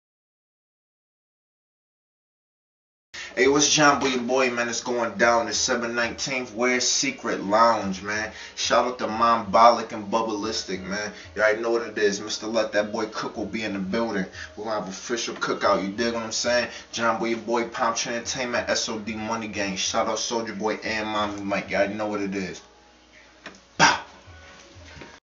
SOULJA BOY'S SODMG ARTIST::JOHN BOY
John Boy speaks about performing LIVE DEC 19th in New York City